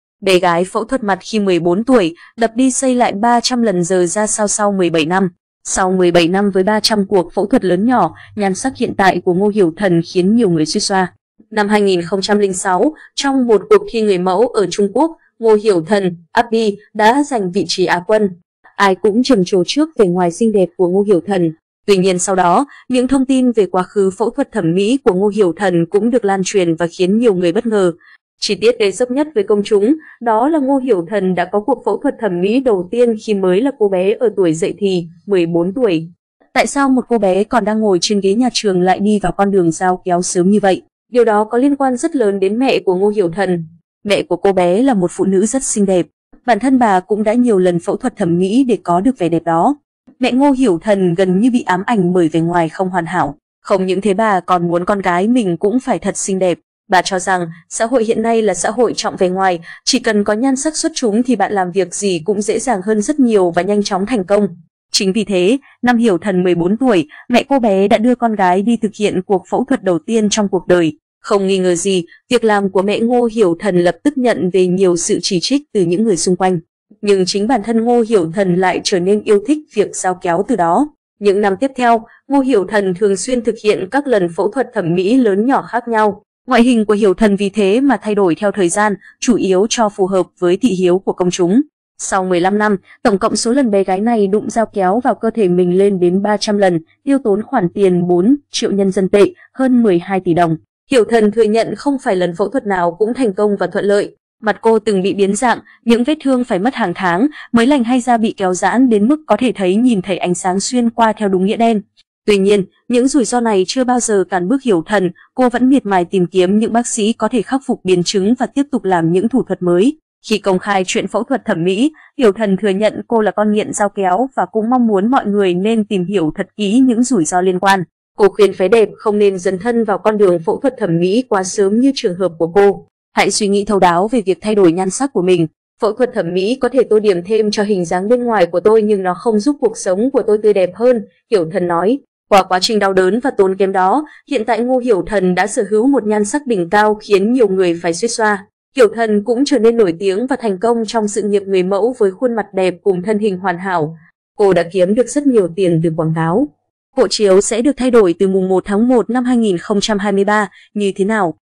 Bé gái phẫu thuật mặt khi 14 tuổi, 'đập đi xây lại' 300 lần giờ ra sao sau 17 năm?
Sau 17 năm với 300 cuộc phẫu thuật lớn nhỏ, nhan sắc hiện tại của Ngô Hiểu Thần khiến nhiều người xuýt xoa.Khi công khai chuyện phẫu thuật thẩm mỹ, Hiểu Thần thừa nhận cô là 'con nghiện' dao kéo và cũng mong muốn mọi người nên tìm hiểu thật kỹ những rủi ro liên quan. Cô khuyên phái đẹp không nên 'dấn thân' vào con đường phẫu thuật thẩm mỹ quá sớm như trường hợp của cô. 'Hãy suy nghĩ thấu đáo về việc thay đổi nhan sắc của mình. Phẫu thuật thẩm mỹ có thể tô điểm thêm cho hình dáng bên ngoài của tôi nhưng nó không giúp cuộc sống của tôi tươi đẹp hơn', Hiểu Thần nói.
Không những thế bà còn muốn con gái mình cũng phải thật xinh đẹp. Bà cho rằng, xã hội hiện nay là xã hội trọng vẻ ngoài, chỉ cần có nhan sắc xuất chúng thì bạn làm việc gì cũng dễ dàng hơn rất nhiều và nhanh chóng thành công.
Chính vì thế, năm Hiểu Thần 14 tuổi, mẹ cô bé đã đưa con gái đi thực hiện cuộc phẫu thuật đầu tiên trong cuộc đời. Không nghi ngờ gì, việc làm của mẹ Ngô Hiểu Thần lập tức nhận về nhiều sự chỉ trích từ những người xung quanh. Nhưng chính bản thân Ngô Hiểu Thần lại trở nên yêu thích việc "dao kéo" từ đó.
Những năm tiếp theo, Ngô Hiểu Thần thường xuyên thực hiện các lần phẫu thuật thẩm mỹ lớn nhỏ khác nhau. Ngoại hình của Hiểu Thần vì thế mà thay đổi theo thời gian, chủ yếu cho phù hợp với thị hiếu của công chúng. Sau 15 năm, tổng cộng số lần bé gái này đụng dao kéo vào cơ thể mình lên đến 300 lần, tiêu tốn khoản tiền 4 triệu nhân dân tệ (hơn 12 tỷ đồng).
Điều đó có liên quan rất lớn đến mẹ của Ngô Hiểu Thần. Mẹ của cô bé là một phụ nữ rất xinh đẹp. Bản thân bà cũng đã nhiều lần phẫu thuật thẩm mỹ để có được vẻ đẹp đó. Mẹ Ngô Hiểu Thần gần như bị ám ảnh bởi vẻ ngoài không hoàn hảo.
Ngô Hiểu Thần xinh đẹp lộng lẫy sau phẫu thuật thẩm mỹ.
Chi tiết gây sốc nhất với công chúng đó là Ngô Hiểu Thần đã có cuộc phẫu thuật thẩm mỹ đầu tiên khi mới là cô bé ở tuổi dậy thì - 14 tuổi. Tại sao một cô bé còn đang ngồi trên ghế nhà trường lại đi vào con đường "dao kéo" sớm như vậy?
Qua quá trình đau đớn và tốn kém đó, hiện tại Ngô Hiểu Thần đã sở hữu một nhan sắc đỉnh cao khiến nhiều người phải xuýt xoa. Hiểu Thần cũng trở nên nổi tiếng và thành công trong sự nghiệp người mẫu với khuôn mặt đẹp cùng thân hình hoàn hảo. Cô đã kiếm được rất nhiều tiền từ quảng cáo.
Hộ chiếu sẽ được thay đổi từ 1.1.2023 như thế nào
Năm 2006, trong một cuộc thi người mẫu ở Trung Quốc, Ngô Hiểu Thần (Abby) đã giành vị trí Á quân. Ai cũng trầm trồ trước vẻ ngoài xinh đẹp của Ngô Hiểu Thần. Tuy nhiên sau đó, những thông tin về quá khứ phẫu thuật thẩm mỹ của Ngô Hiểu Thần cũng được lan truyền và khiến nhiều người bất ngờ.
Hiểu Thần thừa nhận không phải lần phẫu thuật nào cũng thành công và thuận lợi. Mặt cô từng bị biến dạng, những vết thương phải mất hàng tháng mới lành hay da bị kéo giãn đến mức có thể thấy nhìn thấy ánh sáng xuyên qua theo đúng nghĩa đen. Tuy nhiên, những rủi ro này chưa bao giờ 'cản bước' Hiểu Thần, cô vẫn miệt mài tìm kiếm những bác sĩ có thể khắc phục biến chứng và tiếp tục làm những thủ thuật mới.
Nhan sắc của Ngô Hiểu Thần trước khi phẫu thuật thẩm mỹ.